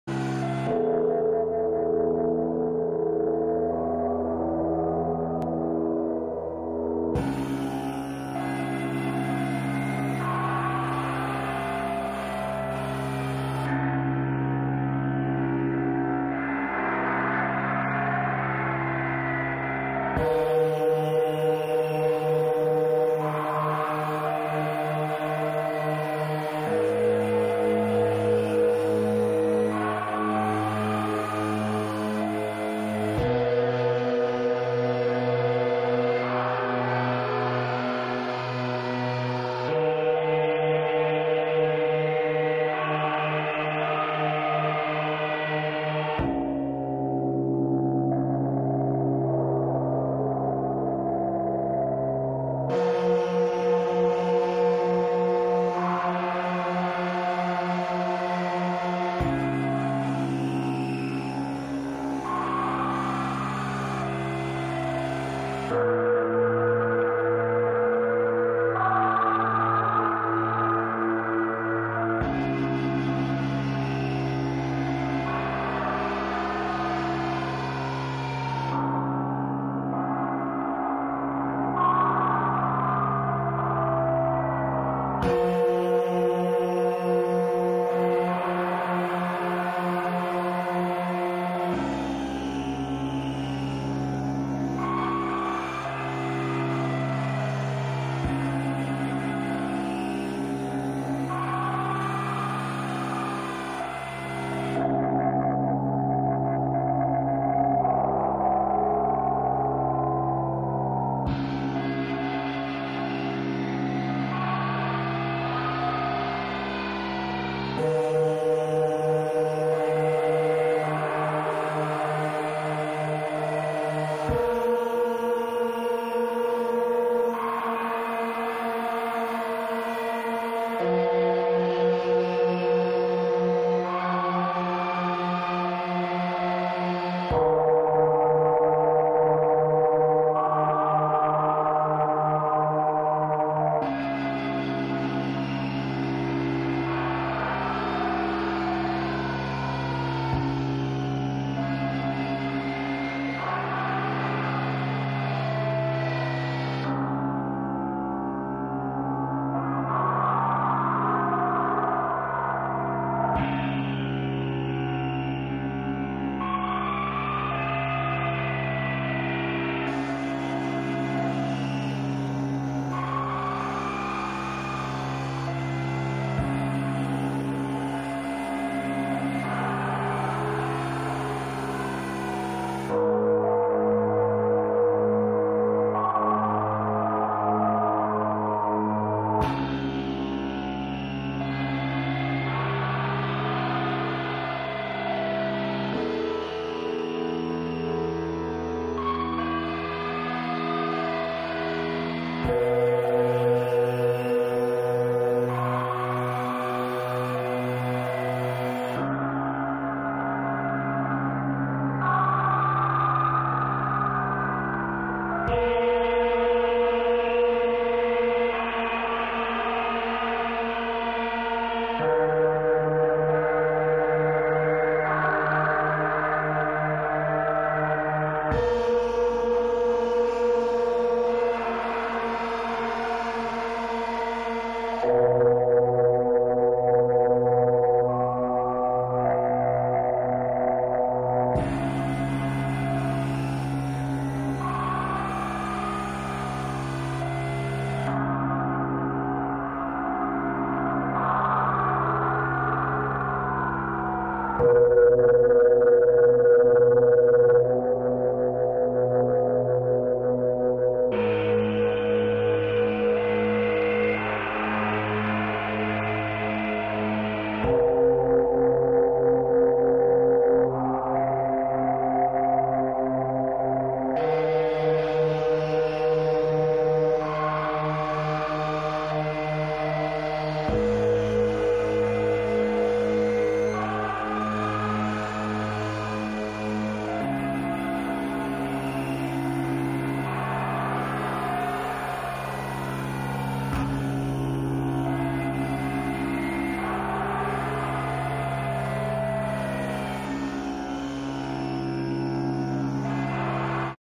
Recursive Machine + TTSH
A short sound demo of the Recursive Machine, processed through the TTSH.   Many thanks to The Human Comparator for creating such awesome machines. These are all diy units built by myself.